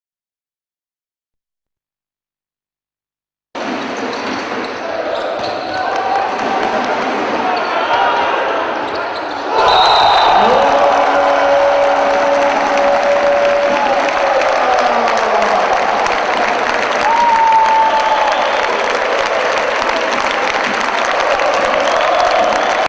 NBA zakucavanje Jure Bobana
KK Alkar : KK Vrijednosnice Osijek (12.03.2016)

Rezultat: 77:68

Autor videa:

Tino Vinković (AB foto Sinj)